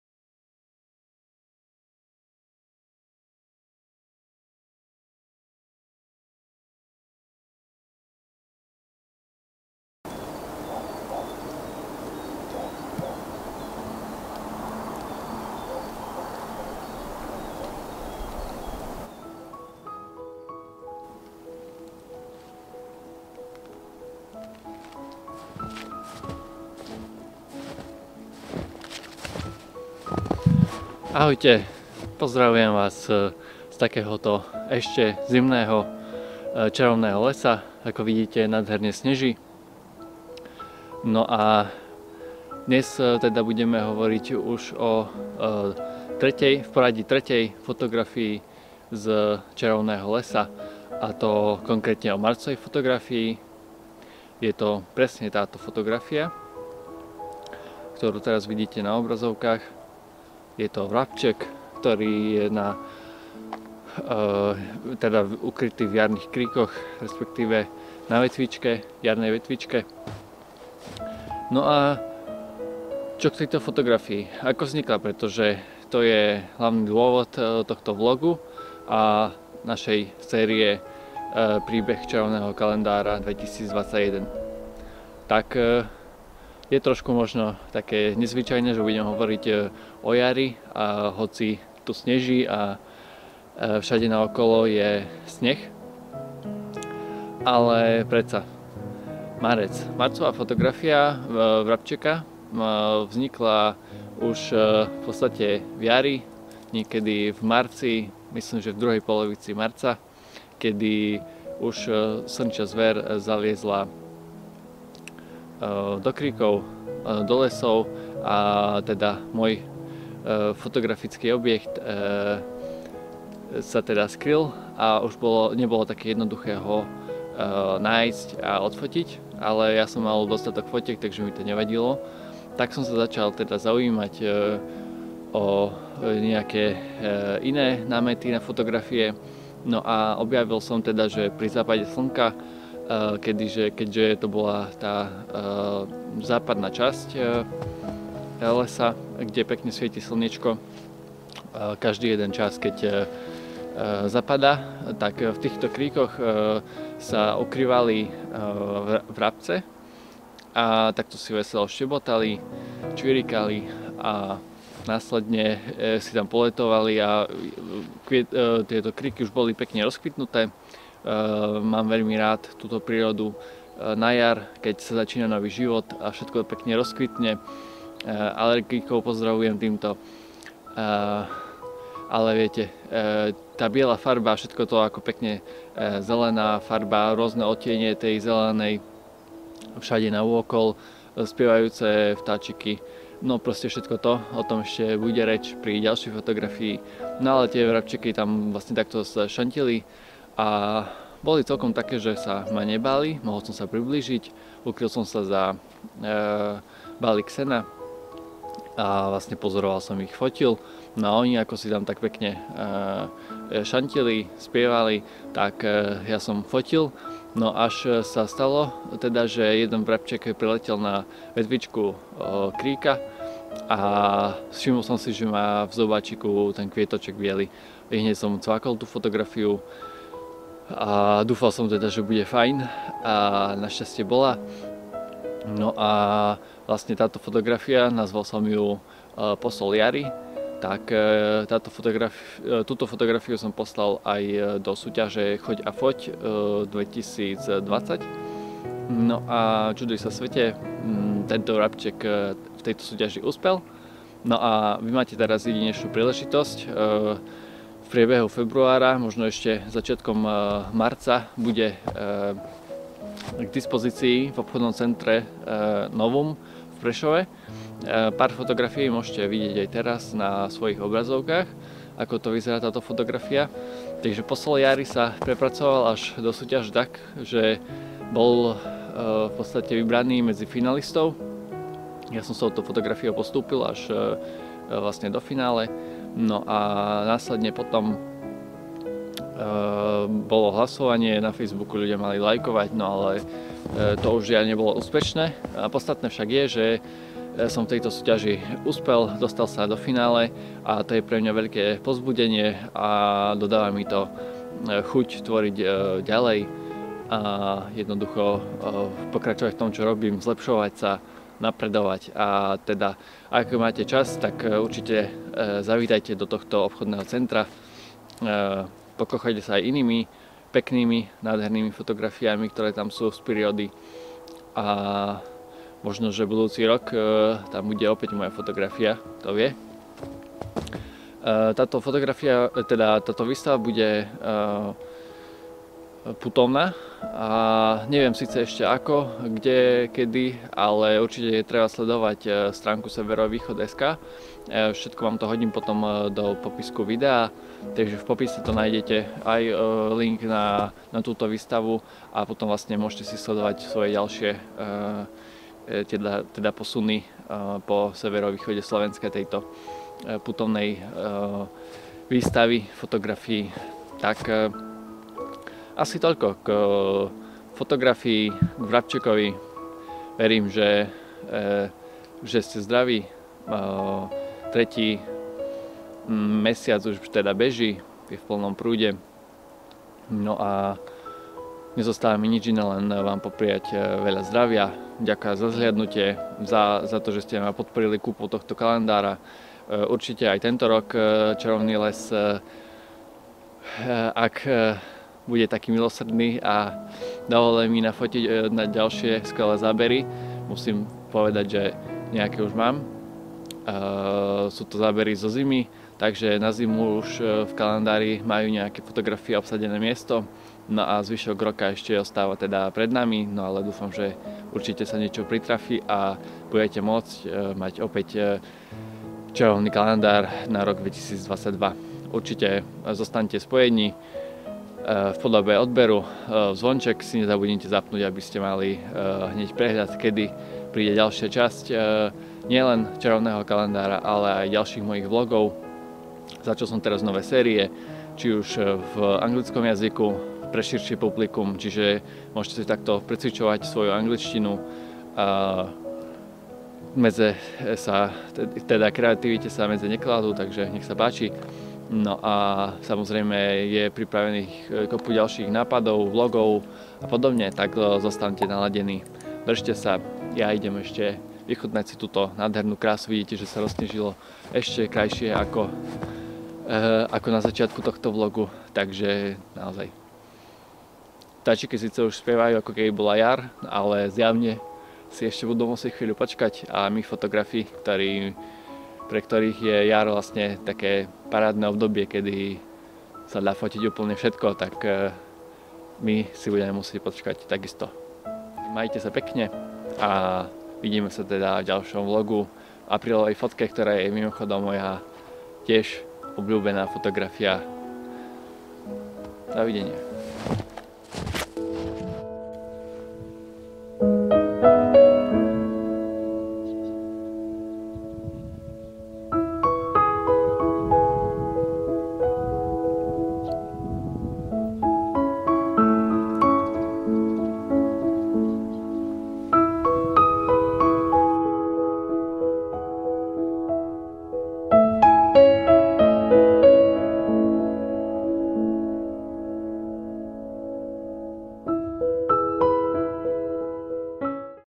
MAREC - VRABEC POĽNÝ | Príbeh Čarovného Kalendára 2021
Pozdravujem zo zasneženého Čarovného lesa! Zima už definitívne stráca na sile a jar pomaly, ale isto klope na dvere, hoci v dnešnom videu ešte nasneží. To však neznamená, že nemôžeme hovoriť o marcovej fotografii z Čarovného kalendára. Je tu už tretí mesiac, takže nezabudnite otočiť stránku  kalendára a užite si toto video!
Ďakujem za podporu!

Greetings from the snowy Magic Forest! Winter is definitely losing its power and spring is already knocking on the door, however it's still snowing in today's video, but that doesn't mean we can't talk about March's photo from the Magic Calendar. It's the third month already, so don't forget to turn the calendar page and enjoy this video!

The magic wood will now be even closer to you!

Čarovný les bude teraz k tebe ešte bližšie!